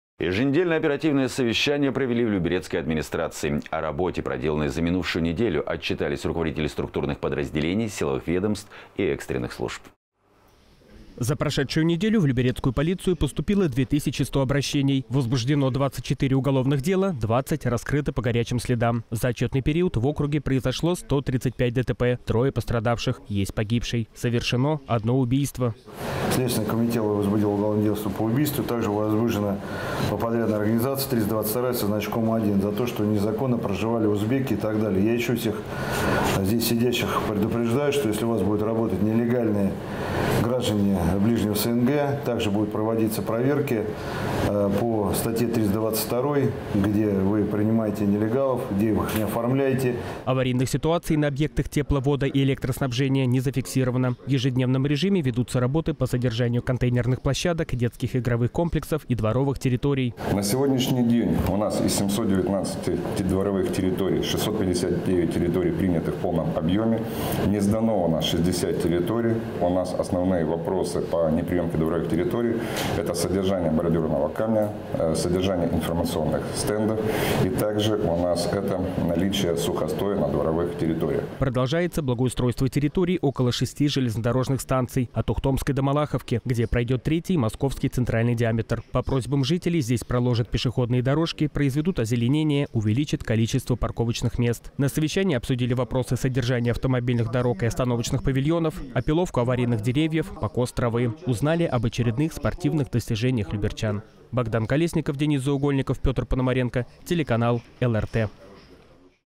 Еженедельное оперативное совещание в администрации
Еженедельное оперативное совещание провели в люберецкой администрации. О работе, проделанной за минувшую неделю, отчитались руководители структурных подразделений, силовых ведомств и экстренных служб.

За прошедшую неделю в люберецкую полицию поступило 2100 обращений, возбуждено 24 уголовных дела, 20 раскрыто по горячим следам. За отчётный период произошло 135 ДТП, трое пострадавших, есть погибший. Совершено одно убийство.

Евгений Романцев, начальник МУ МВД России «Люберецкое»: «Следственный комитет возбудил уголовное дело по убийству. Также возбуждено уголовное дело по подрядной организации: 132-я со значком 11. То, что незаконно проживали узбеки и так далее. Я ещё раз предупреждаю, что если у вас будут работать нелегально граждане ближнего СНГ, также будут проводиться проверки по статье 322-й, где вы принимаете нелегалов, их не оформляете».

Аварийных ситуаций на объектах тепло-, водо- и электроснабжения не зафиксировано. В ежедневном режиме ведутся работы по содержанию контейнерных площадок, детских игровых комплексов и дворовых территорий.

Василий Власов, заместитель главы администрации г.о. Люберцы: «На сегодняшний день у нас 719 дворовых территорий 659 – принято в полном объёме. Не сдано 60 территорий. Основные вопросы неприёмки дворовых территорий – это содержание бордюрного камня, информационных стендов и наличие сухостоя на дворовых территориях. На сегодняшний день с данными управляющими компаниями и ГУСТом полностью согласована дорожная карта. Она расписана по каждому адресу».

Продолжается благоустройство территорий около шести железнодорожных станций – от Ухтомской до Малаховки, где пройдёт 3-й Московский центральный диаметр. По просьбам жителей здесь проложат пешеходные дорожки, произведут озеленение, увеличат количество парковочных мест. На совещании обсудили вопросы содержания автомобильных дорог и остановочных павильонов, опиловку аварийных деревьев, покос травы. Узнали об очередных спортивных достижениях люберчан.